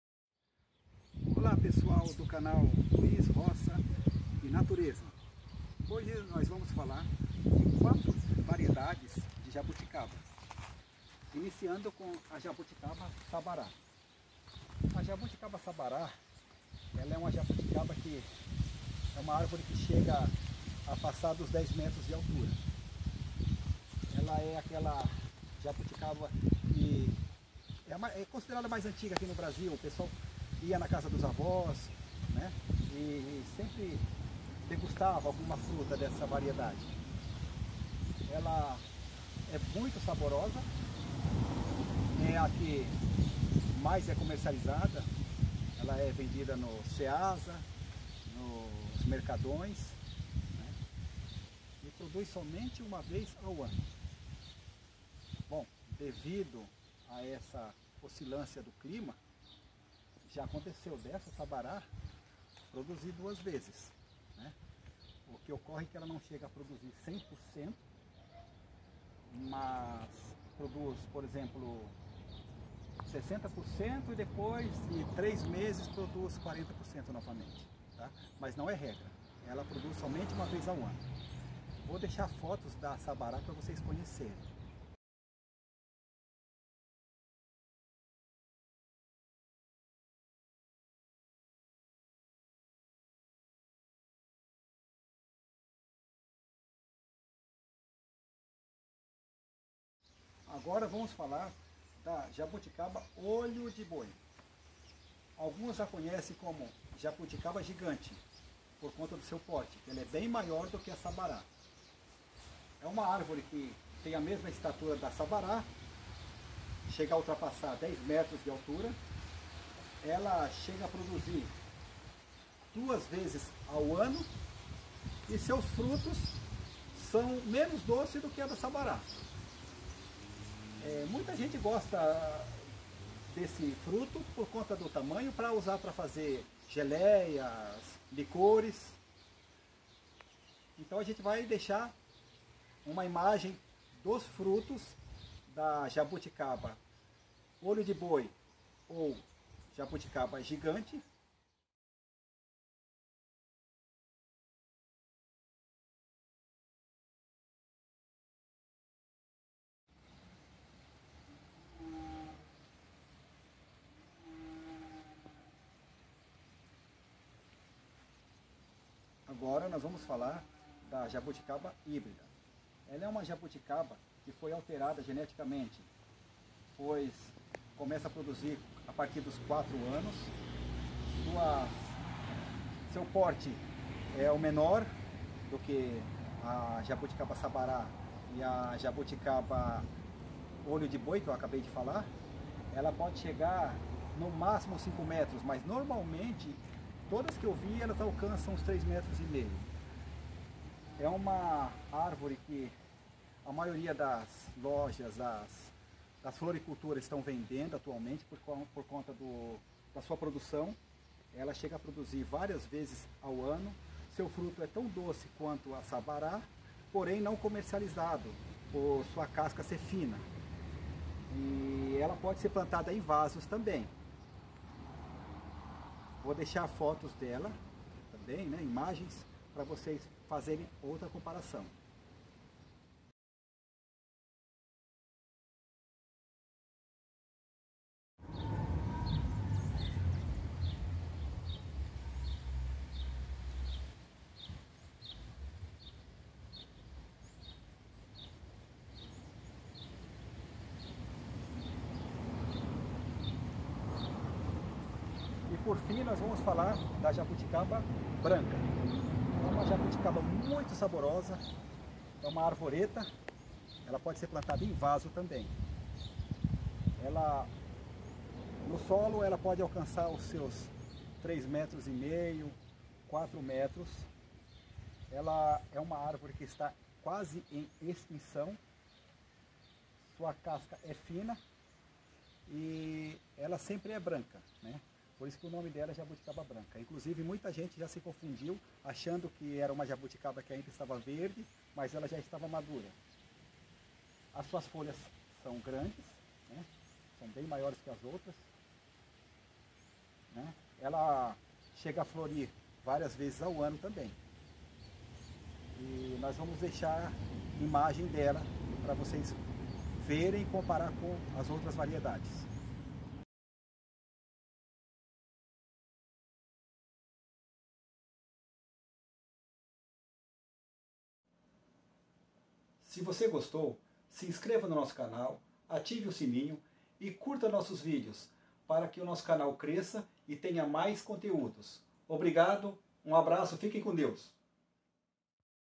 Jabuticaba em Pomerode SC, 4 variedades
simples comparação entre jabuticabas , sabará, olho de Boi ( gigante ), hibrida e jabuticaba branca .